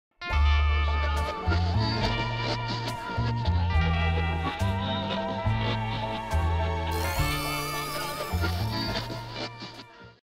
Bonanza Bonanza TV Sting 01
Some more of our work for the very excellent SKY TV Channel 266...Bonanza Bonanza.